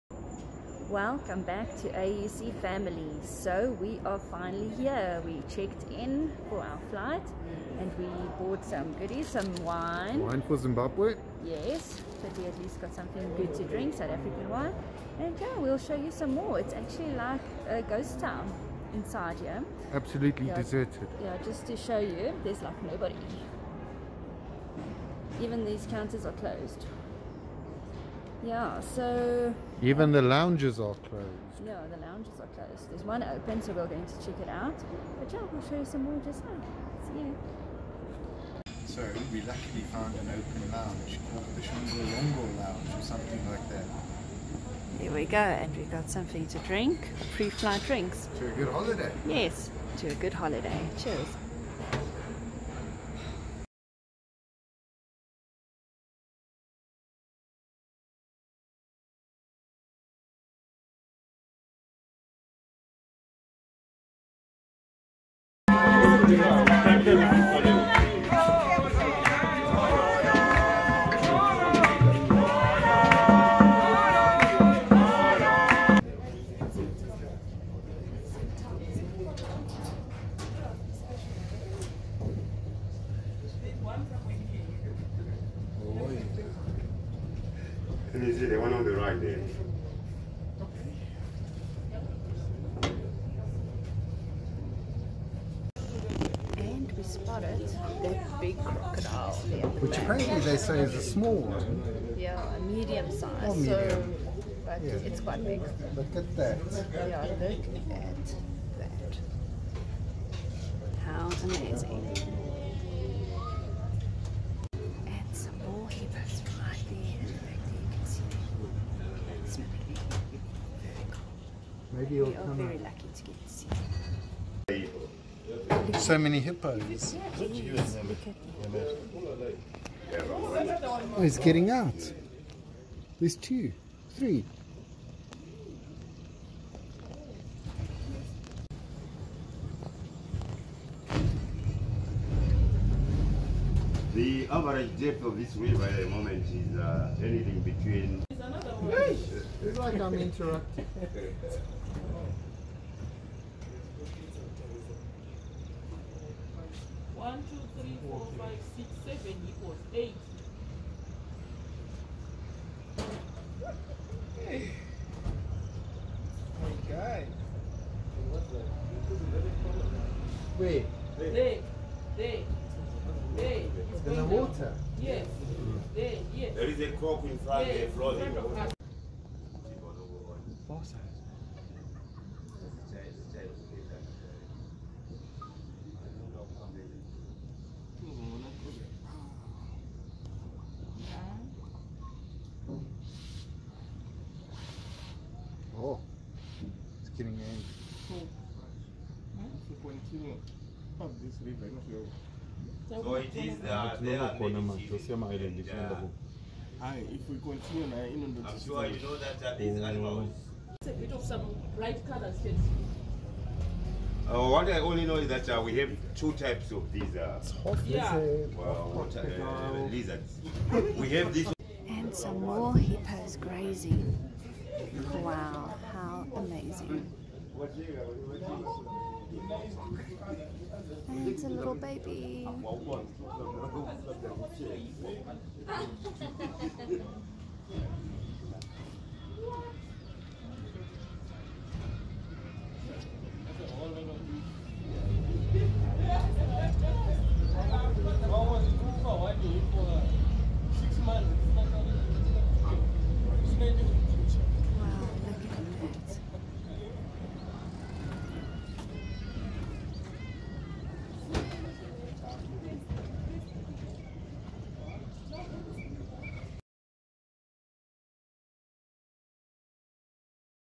First International Trip since Covid-19
Today mom and dad took their first trip since Covid-19 happened. The international departure at the airport is still a ghost town. The flight was almost empty. Arrived in Victoria Falls and did a sunset cruise with lots of animal sightings.